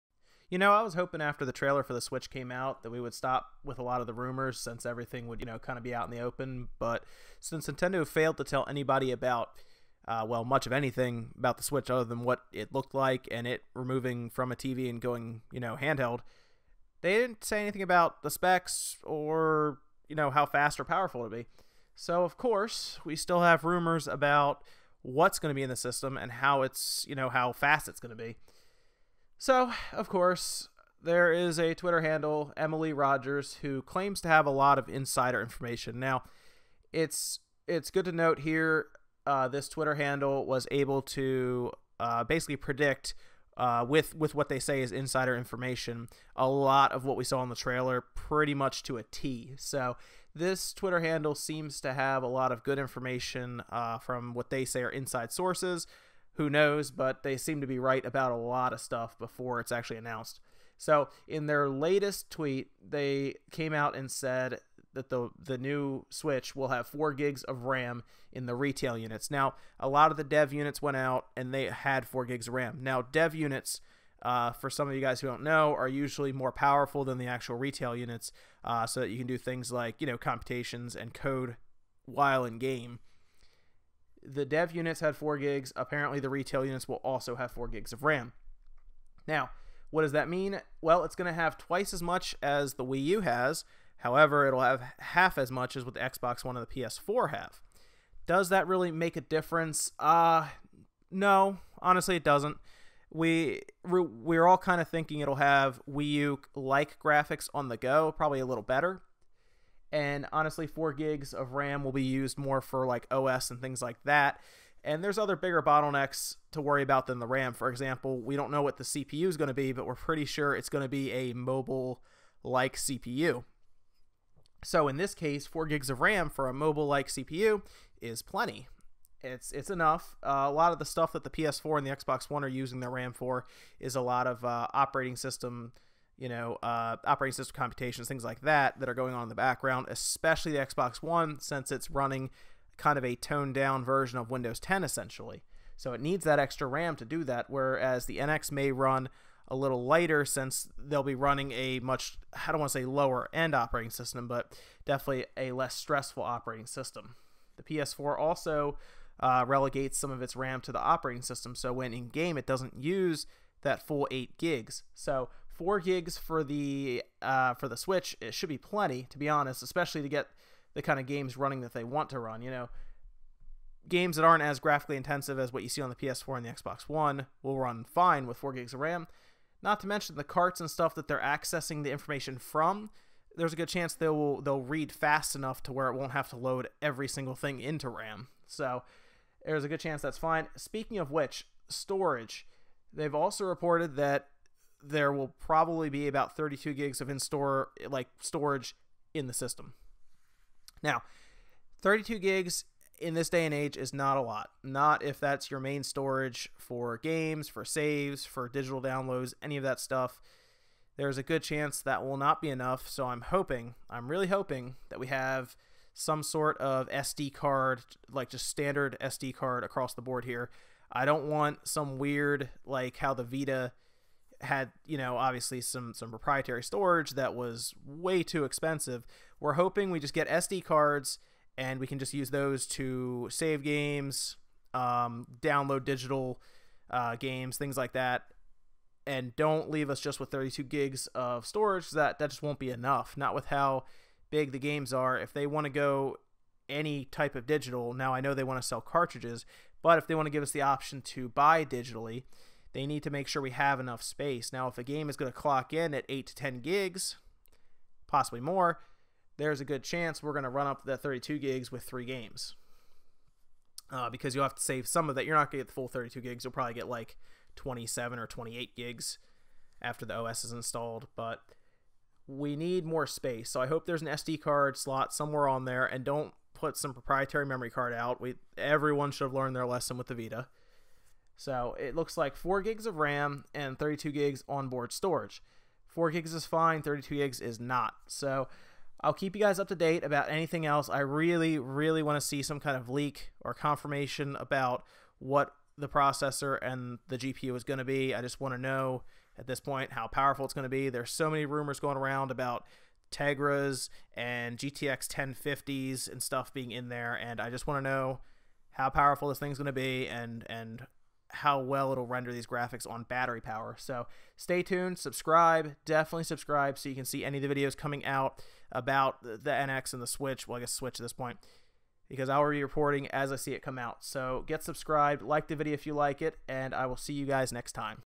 Nintendo Switch Has 4GB of Ram??
New leaks put the Nintendo Switch with 4GB of ram. That would give the Switch twice as much ram as the Wii U but only half the ram of the PS4 and Xbox one. This won't be as big of a deal considering it will be using a mobile CPU to power the tablet.